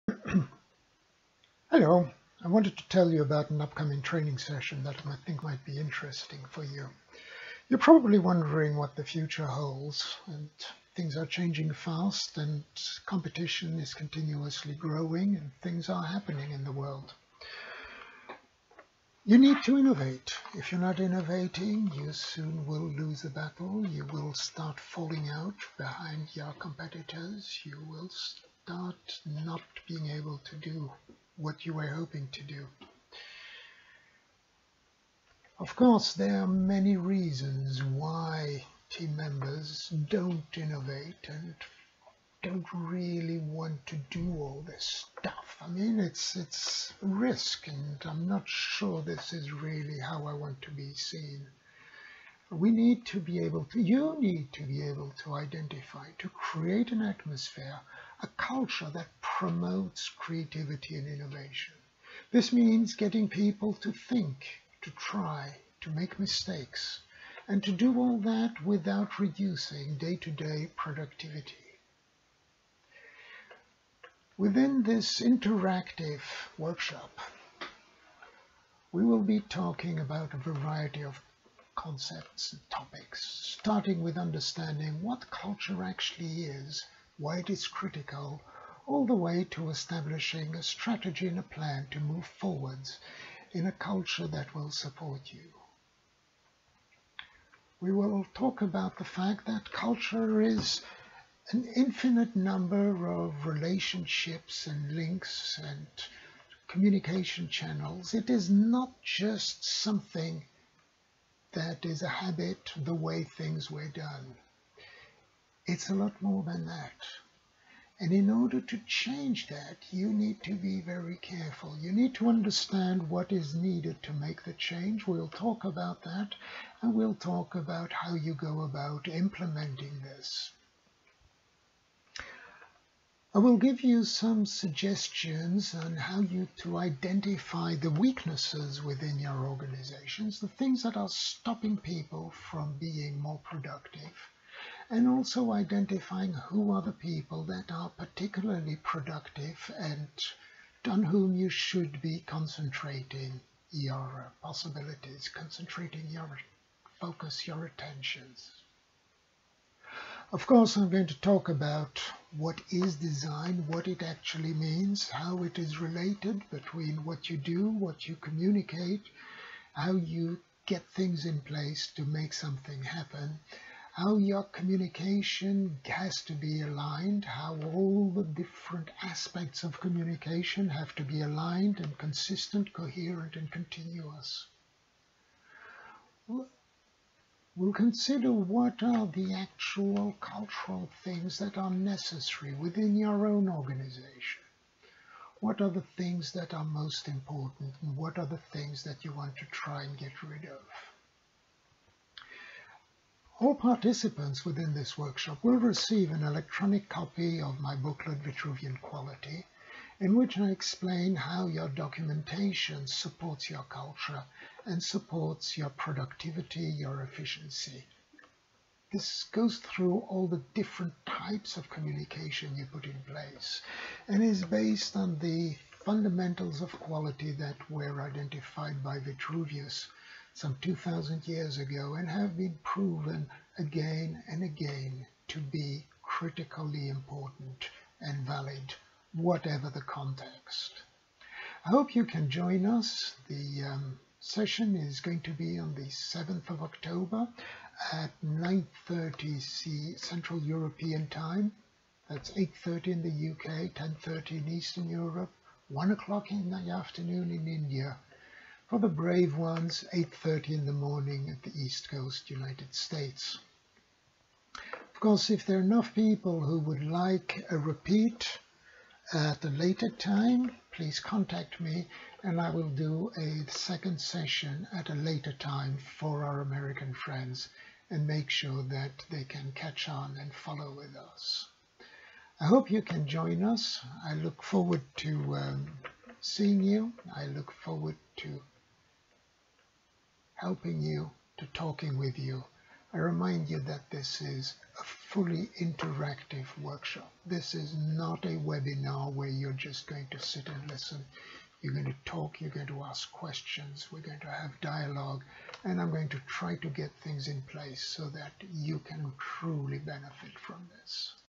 Designing a Culture for Innovation - Interactive online workshop (07 Oct 2020)
You need to innovate to survive in today's world - whether it is new products or services, or a better way of achieving the expected results.
This interactive workshop will guide you through the principles of culture and change management so that you can start implementing the results as soon as possible.

I am delivering the workshop twice, once at 08:00 UTC and once at 20:00 UTC - hopefully, one of these fits your timezone.